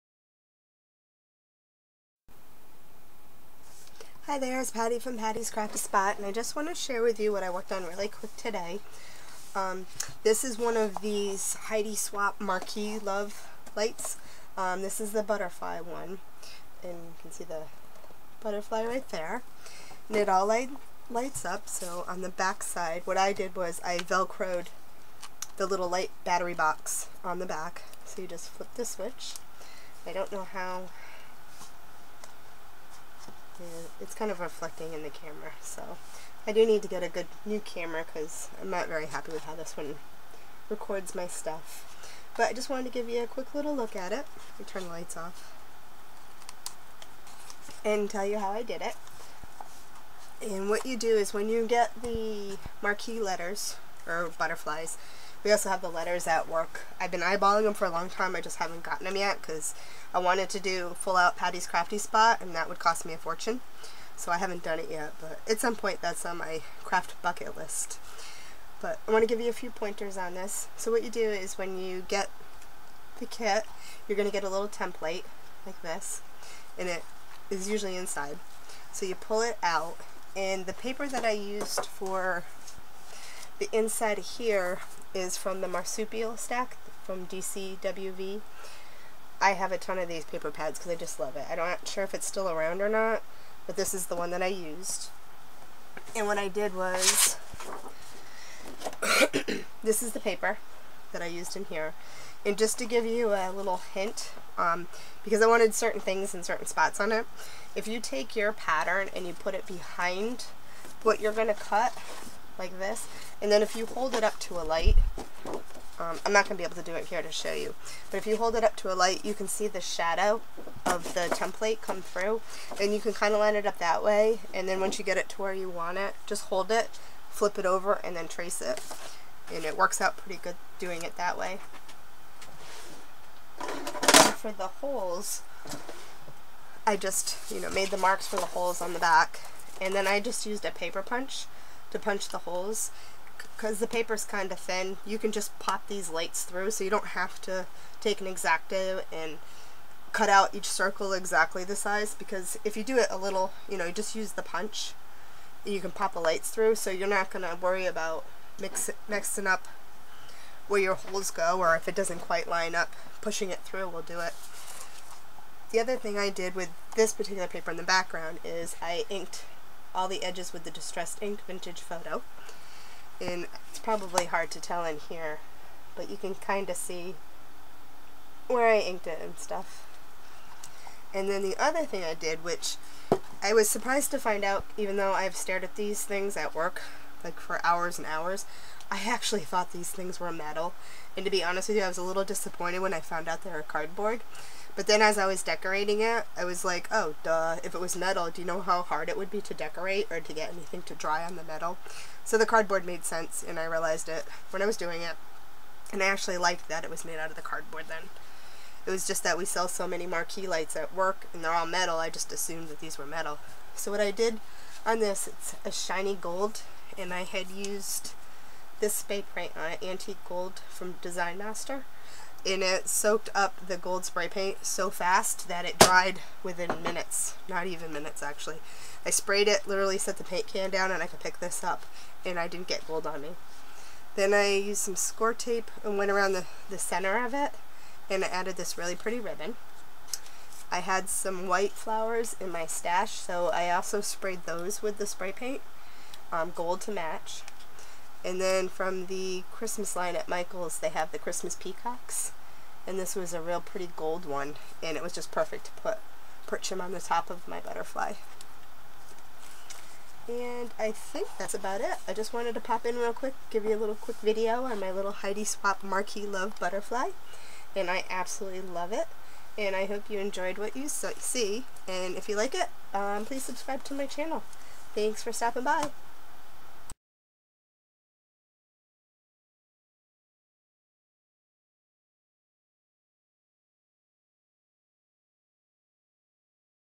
Heidi Swapp Marquee Love Butterfly Light by Pattys Crafty Spot
Heidi Swapp Marquee Love Butterfly Light
Just a quick video with some hints if you haven't made one yet that I found helpful and I hope you do to.

Search for me on Instagram and Periscope
Pattys Crafty Spot

~Patricia~